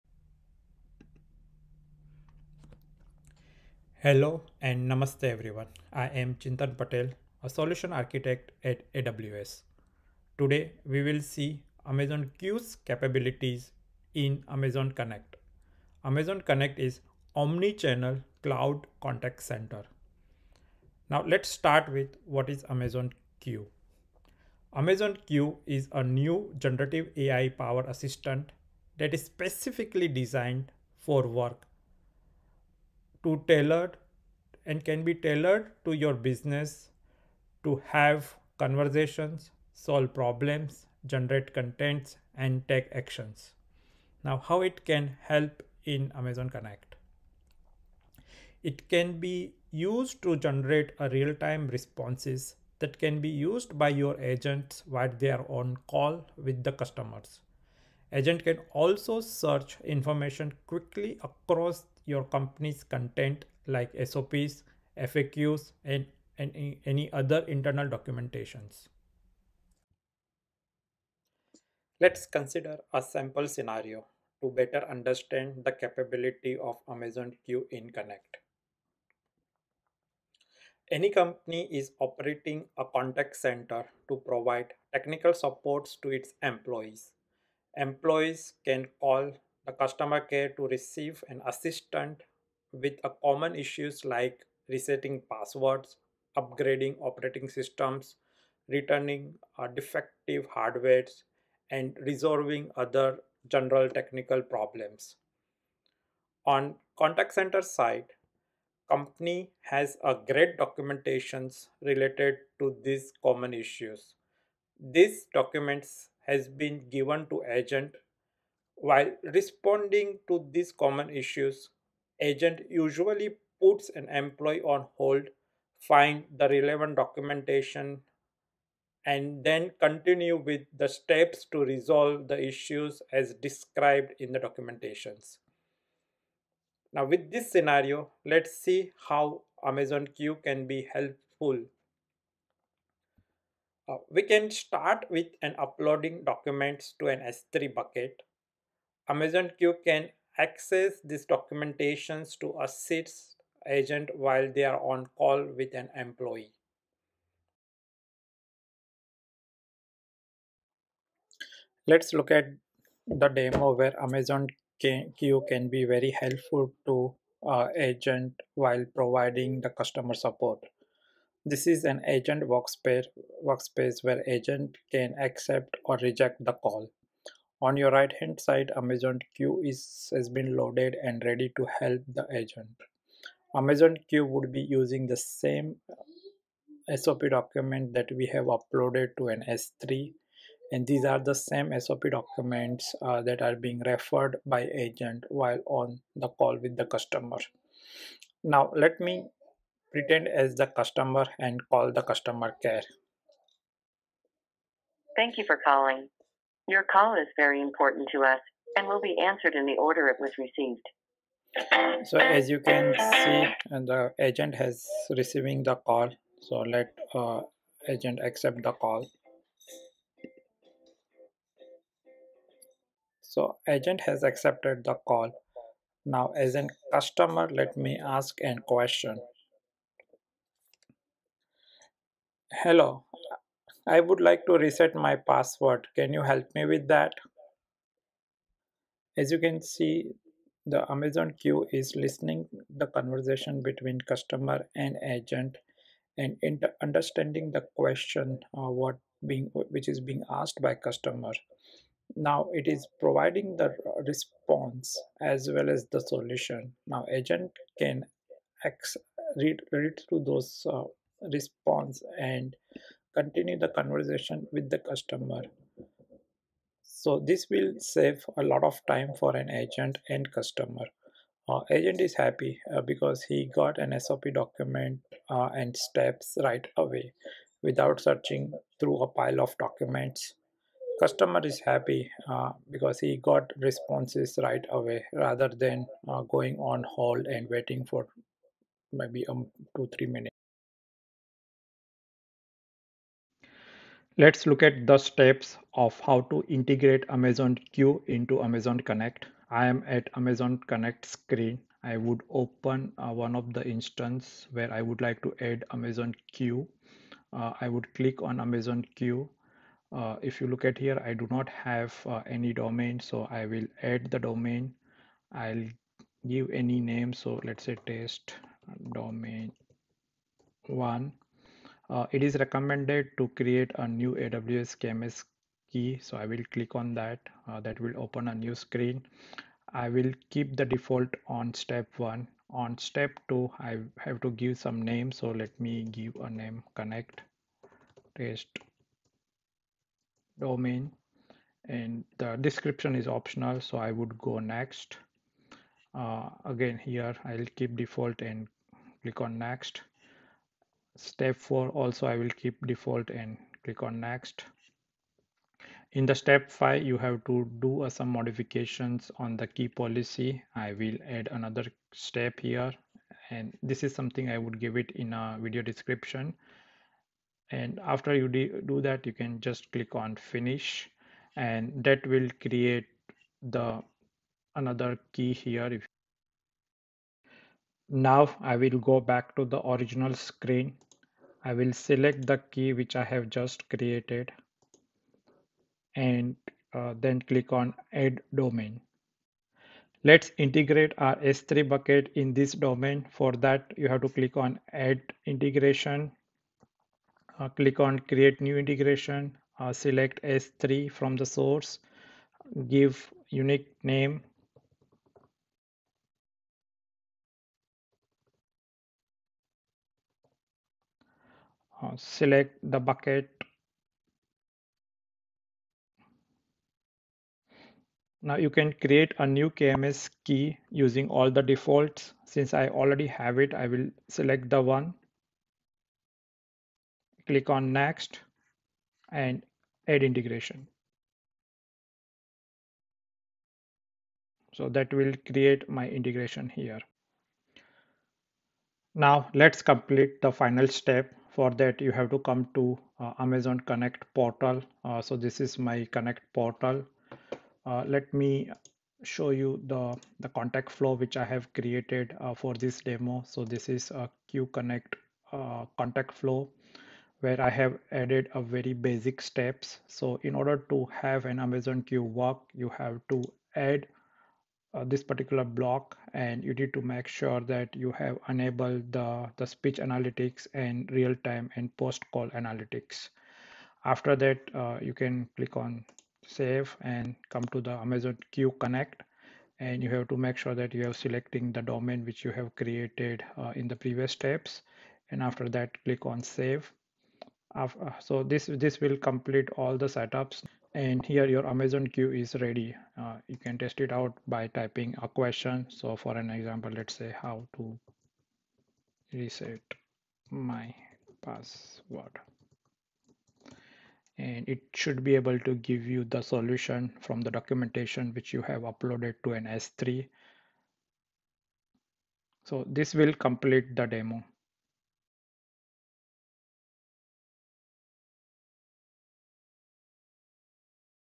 Boost Agent Productivity and Customer Satisfaction through Amazon Q for Connect
It Provides a tutorial on enabling Amazon Q for your Amazon Connect contact center. It covers key concepts like:

- What is Amazon Q for Connect and how it can be used to assist contact center agents
- Steps to enable Amazon Q for Connect, including creating an KMS key and attaching the necessary IAM policy
- Integrating Amazon Q into your contact flows to provide agents with real-time recommendations and assistance

More details on Amazon Q Connect

Please copy below IAM Policy Statement while creating KMS Key as mention in video ( at time 06:00 )
            "Effect": "Allow",
            "Principal": {
            "Action": [
                "kms:Decrypt",
                "kms:GenerateDataKey*",
                "kms:DescribeKey"